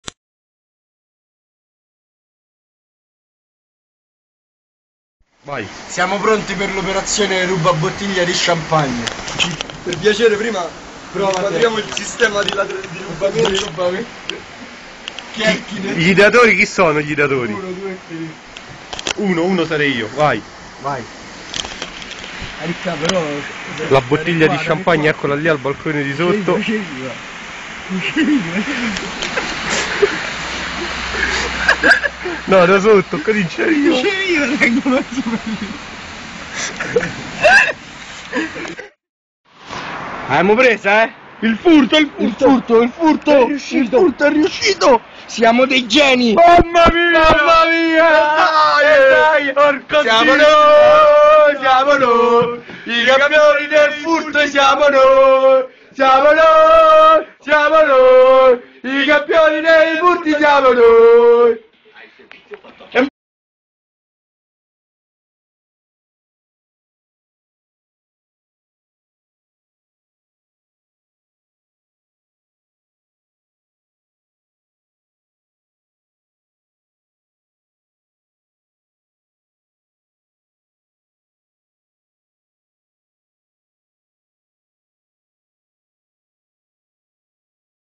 Il Tordo ladro (uccello mitologico)
Un gruppo di animali mitologici cerca di rubare una bottiglia di (presunto) champagne...